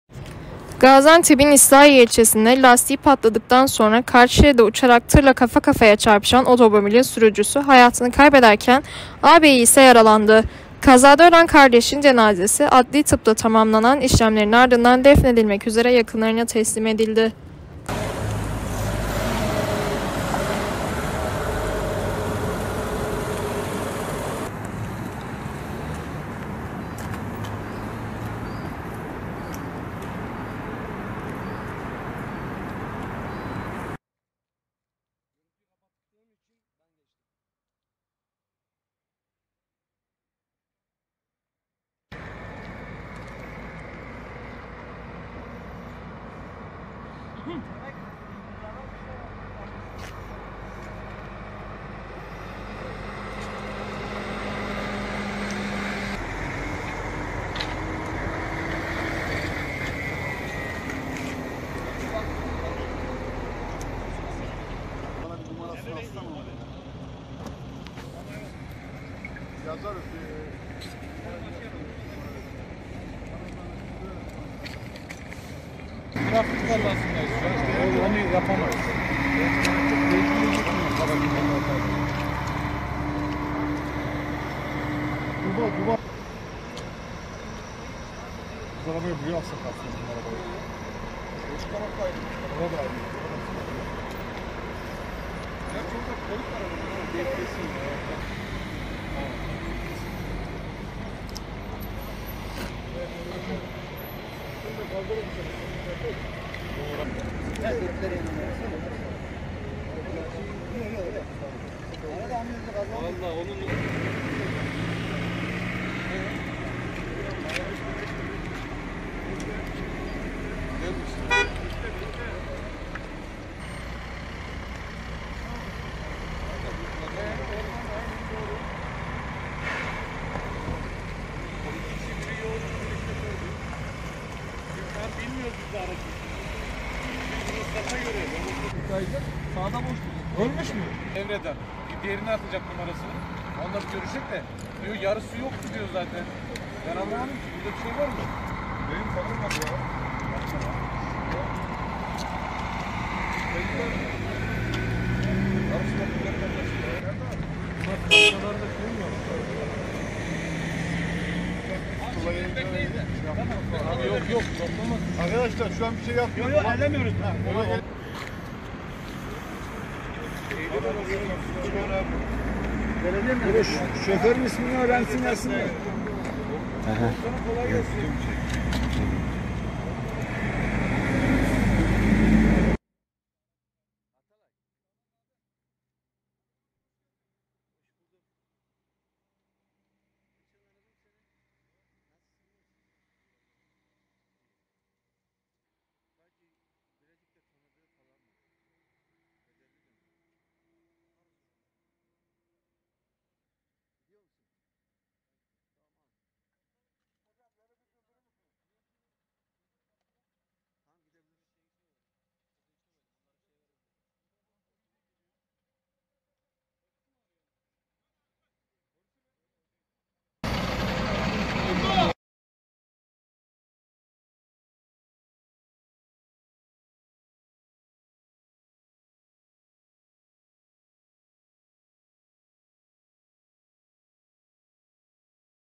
Feci Kaza Ağabey İle Kardeşi Ayırdı
Gaziantep’in İslahiye ilçesinde lastiği patladıktan sonra karşı şeride uçarak tırla kafa kafaya çarpışan otomobilin sürücüsü hayatını kaybederken, ağabeyi ise yaralandı. Kazada ölen kardeşin cenazesi Adli Tıp'ta tamamlanan işlemlerinin ardından defnedilmek üzere yakınlarına teslim edildi.